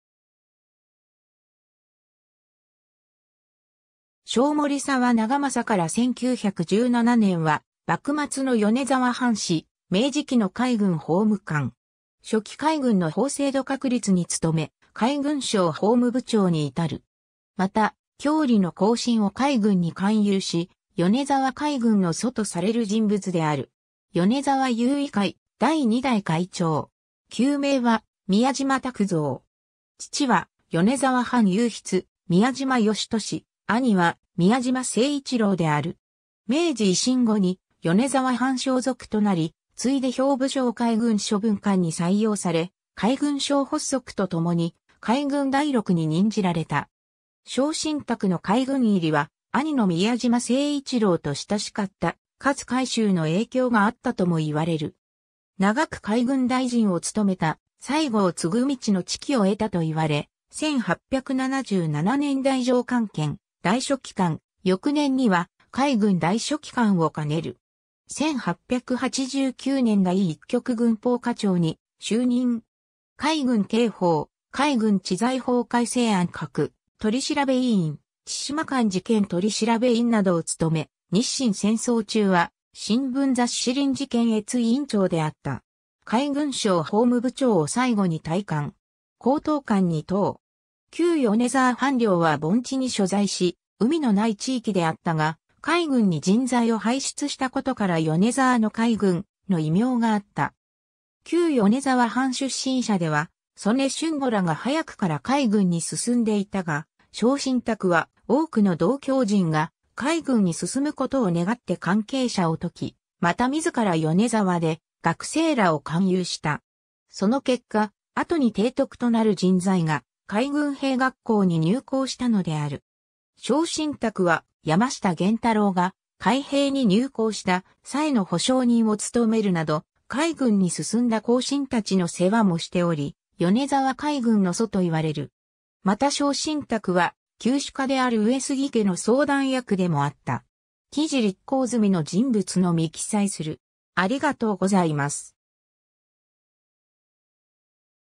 小森沢長政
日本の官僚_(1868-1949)
小森沢 長政（こもりさわ ながまさ、1843年（天保14年） - 1917年（大正6年））は、幕末の米沢藩士、明治期の海軍法務官。
初期海軍の法制度確立に努め、海軍省法務部長に至る。
また郷里の後進を海軍に勧誘し、米沢海軍の祖とされる人物である。
米沢有為会第二代会長。
 旧名は宮島琢蔵。
父は米沢藩右筆・宮島吉利、兄は宮島誠一郎である。
明治維新後に米沢藩少属となり、次いで兵部省海軍所文官に採用され、海軍省発足とともに海軍大録に任じられた。
小森沢の海軍入りは兄の宮島誠一郎と親しかった勝海舟の影響があったともいわれる。
長く海軍大臣を務めた西郷従道の知己を得たといわれ、1877年（明治10年）太政官権大書記官、翌年には海軍大書記官を兼ねる。
1889年（明治22年）第一局軍法課長に就任。
海軍刑法・海軍治罪法改正案各取調委員、千島艦事件取調委員などを務め、日清戦争中は新聞雑誌臨時検閲委員長であった。
海軍省法務部長を最後に退官。
高等官二等。
 旧米沢藩領は盆地に所在し、海のない地域であったが、海軍に人材を輩出したことから"米沢の海軍"の異名があった。
旧米沢藩出身者では曽根俊虎らが早くから海軍に進んでいたが、小森沢は多くの同郷人が海軍に進むことを願って関係者を説き、また自ら米沢で学生らを勧誘した。
その結果、後に提督となる人材が海軍兵学校に入校したのである。
小森沢は山下源太郎（宮島誠一郎の女婿）が海兵に入校した際の保証人を務めるなど、海軍に進んだ後進たちの世話もしており、米沢海軍の祖といわれる。
また小森沢は旧主家である上杉家の相談役でもあった。
 記事立項済みの人物のみ記載する。